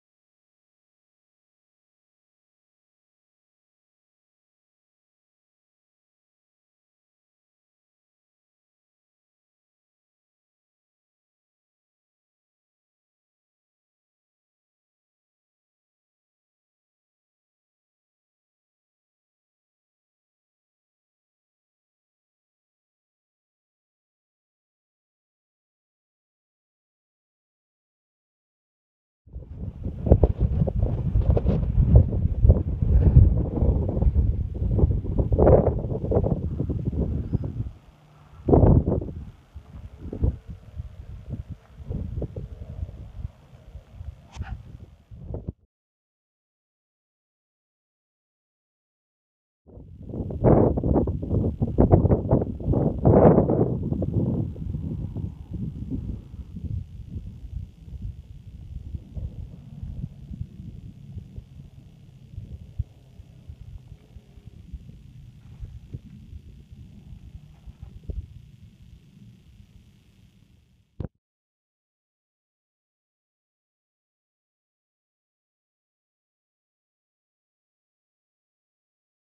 Sears Kay Time Lapse
Watching the 12-13-14 storms roll in over Cave Creek from the Sears-Kay Hohokam Indian Ruins in the Tonto National Forest.